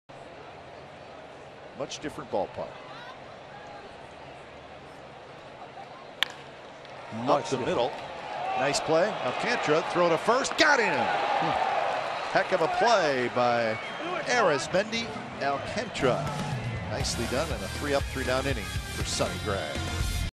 OAK@LAA: Alcantara lays out to make a nice stop
Arismendy Alcantara dives to make the stop on Carlos Perez's grounder up the middle and throws to first base for the out to end the 4th